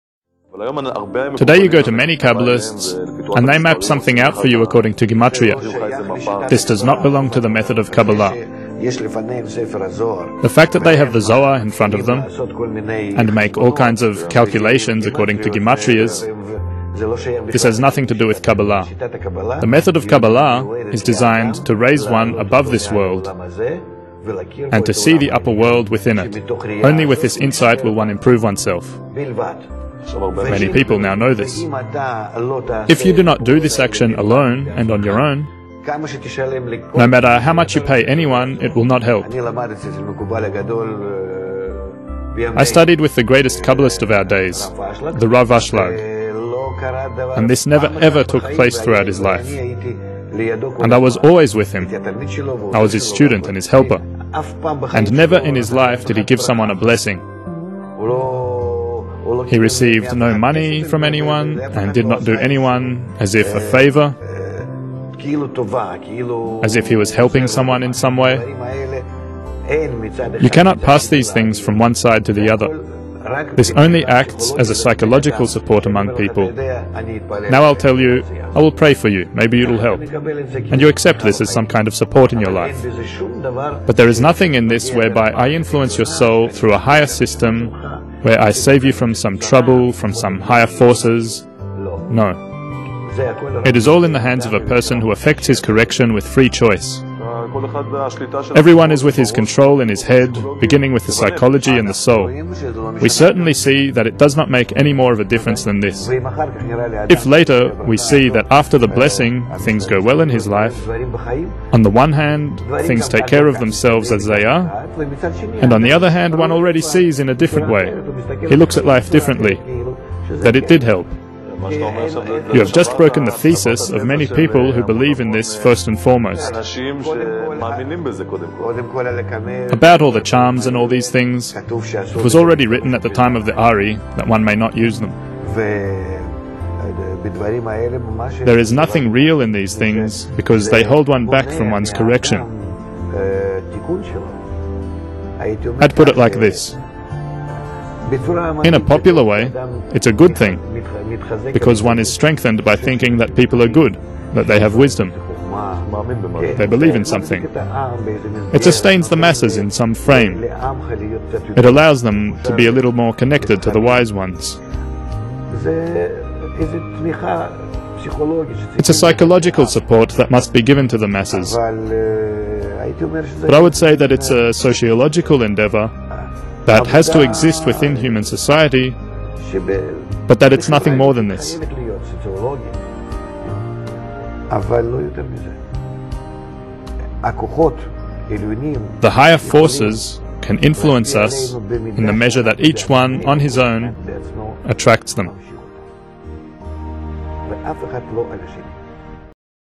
Isto Não é Cabala
- Rav Laitman explica que encantamentos e bênçãos não têm lugar na Cabala. o maior arquivo do mundo em Cabala